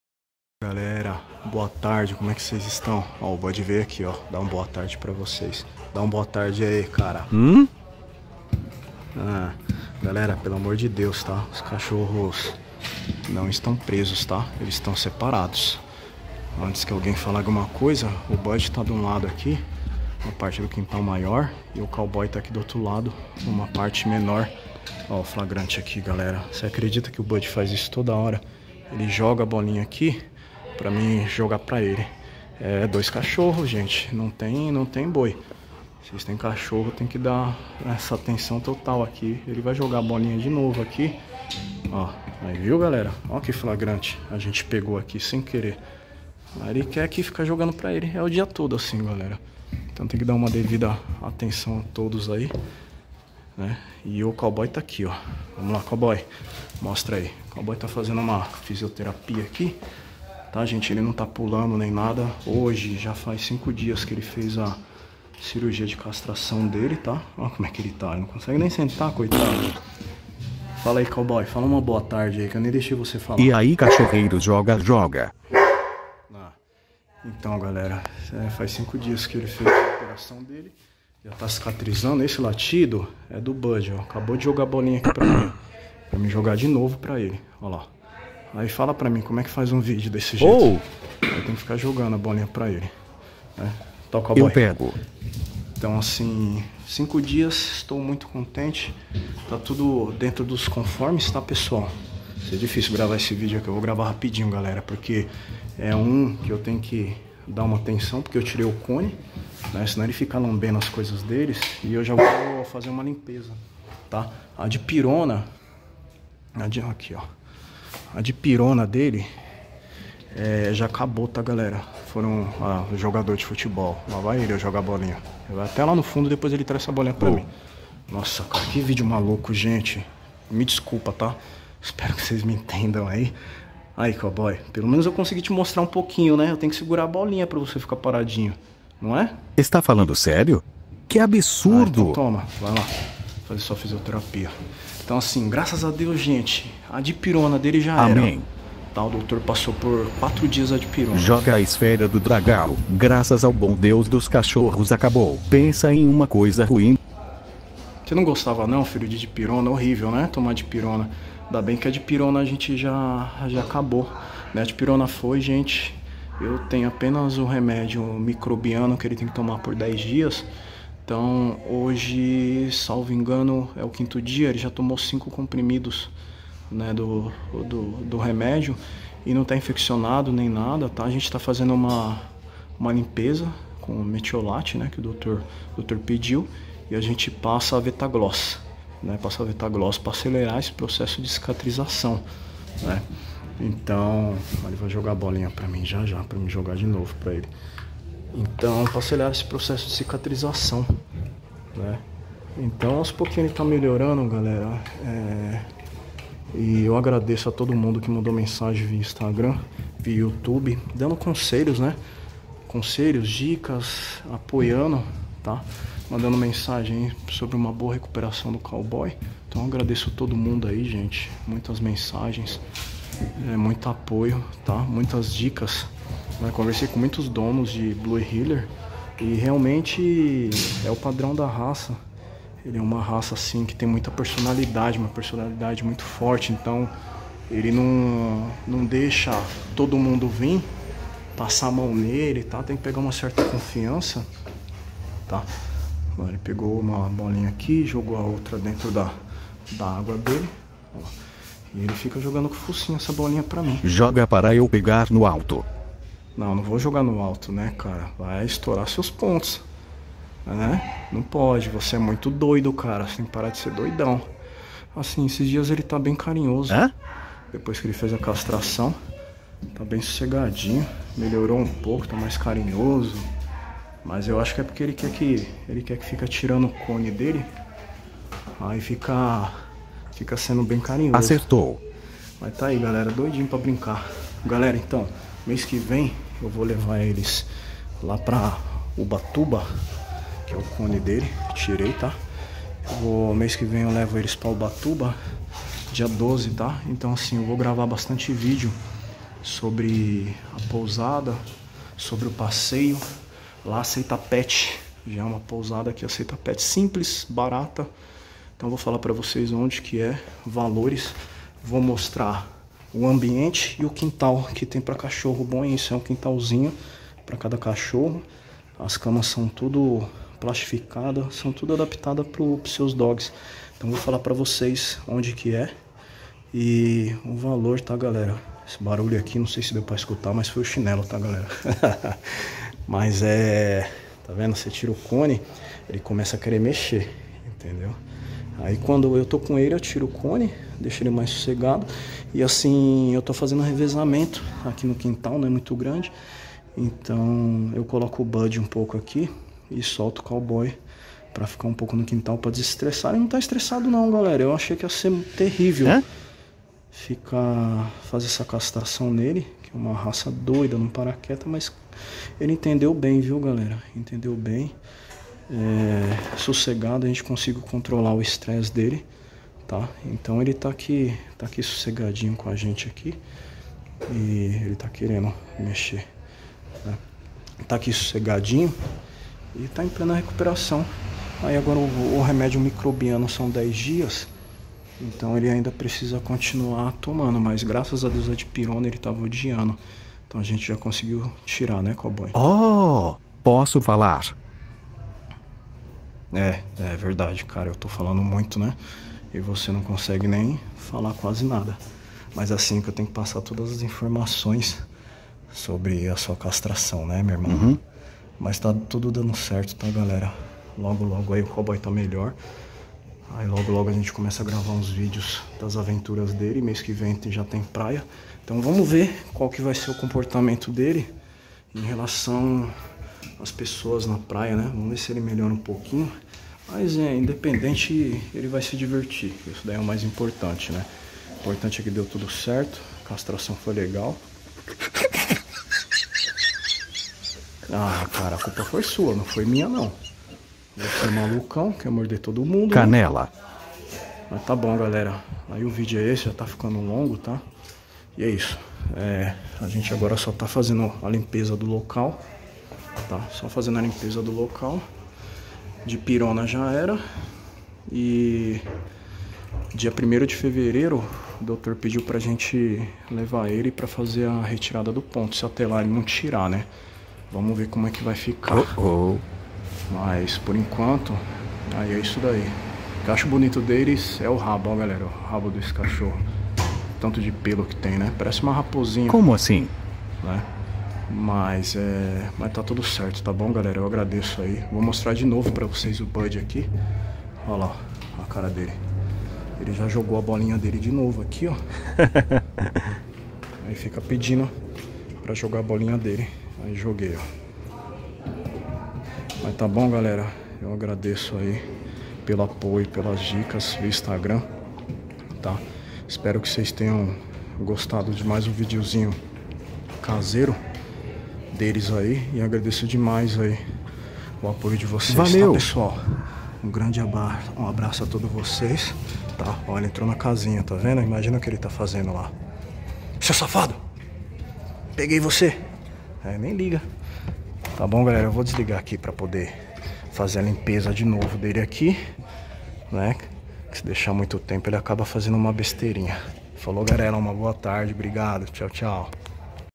O blue heeler está no quinto dia de recuperação pós cirurgia, como estão as coisas🙏🤗🦝🐾
Galera tem vídeo novo sobre a castração do Cowboy como ele está após 5 dias da cirurgia para quem pensa em castrar seu dog assista 🦝🐾🤗🥰🙏